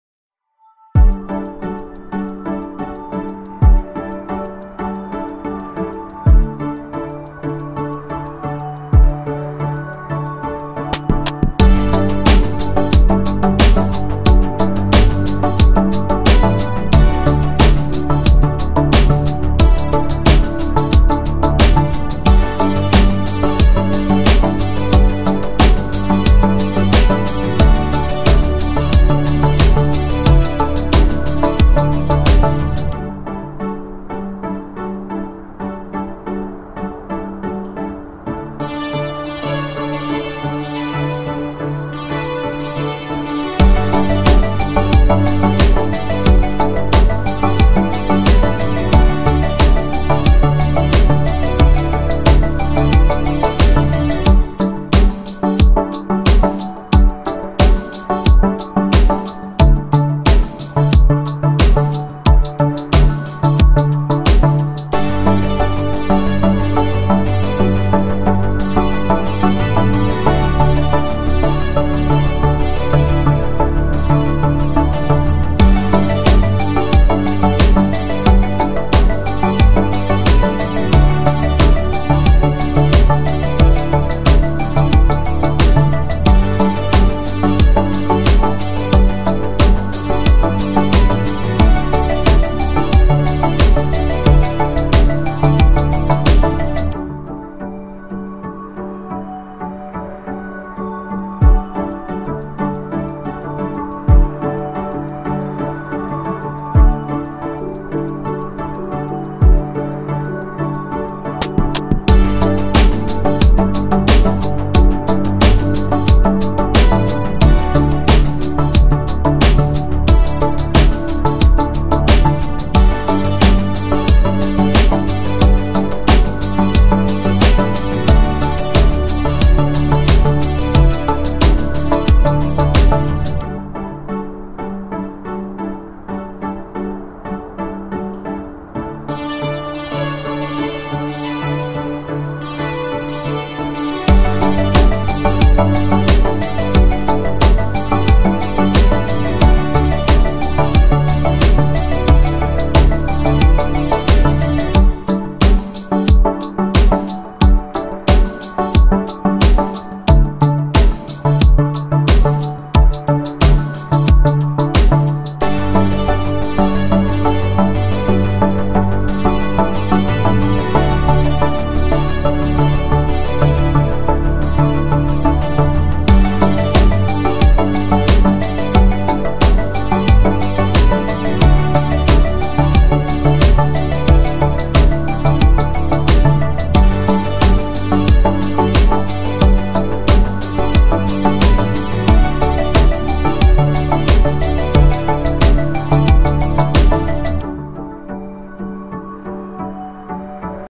how to draw abutterfly by watercolor (wall decor)
You can create a beautiful painting as a wall decor using only watercolor